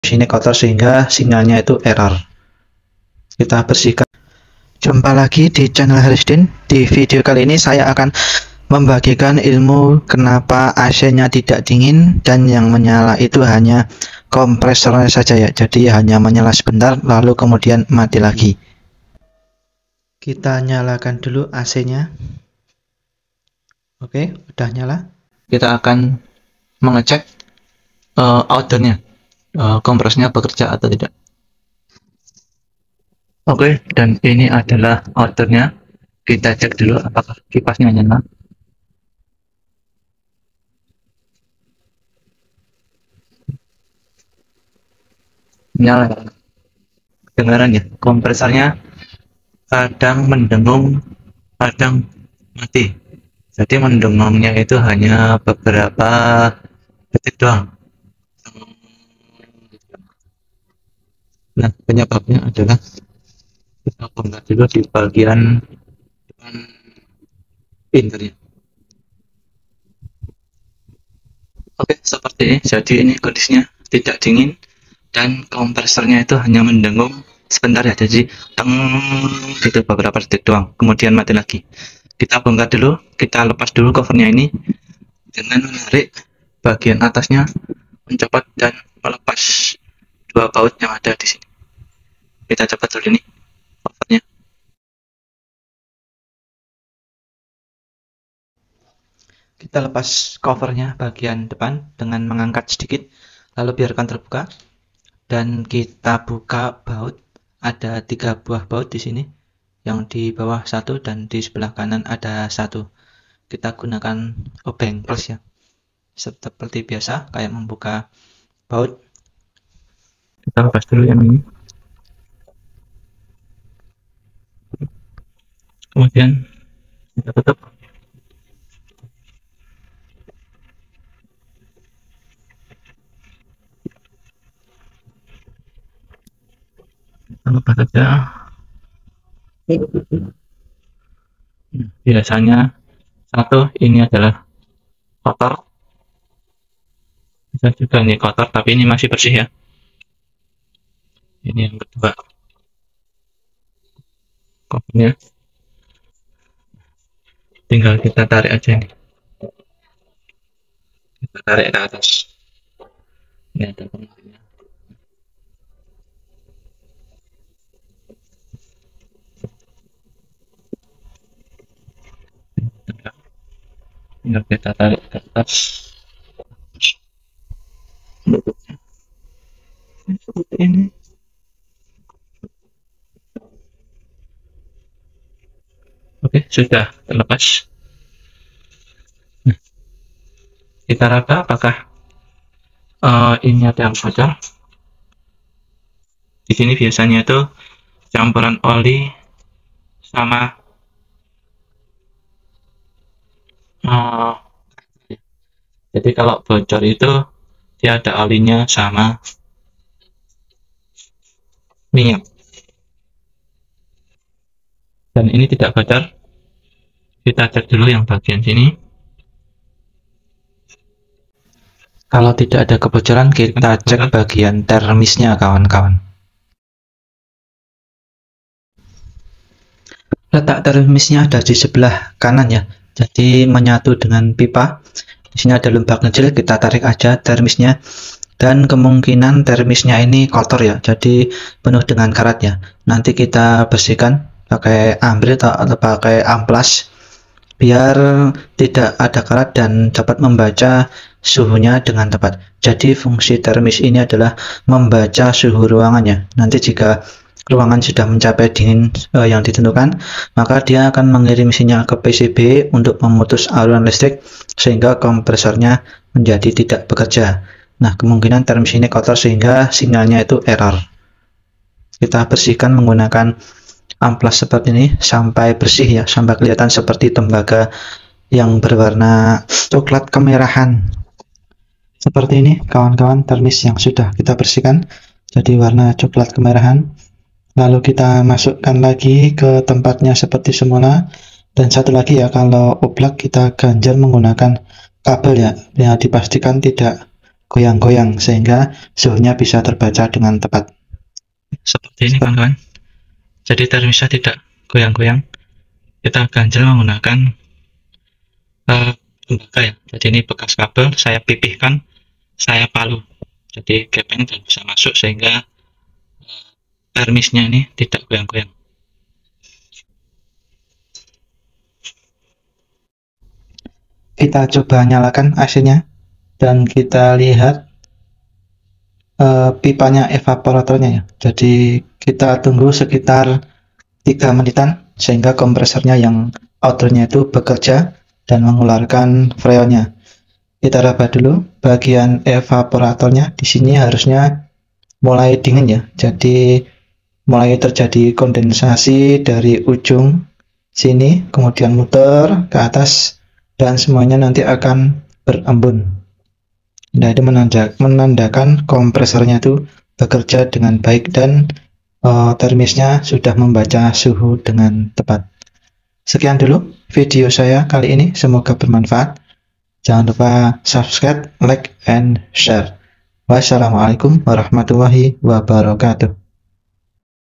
AC tidak dingin Kompresor kadang nyala kadang mati// thermis eror
AC sudah tidak dingin tapi indoor tidak mengalami kebocoran

Kompresor kadang mati kadang nyala

Bisa jadi hanya termisnya yang kotor coba bersihkan bagian thermisnya

semoga berhasil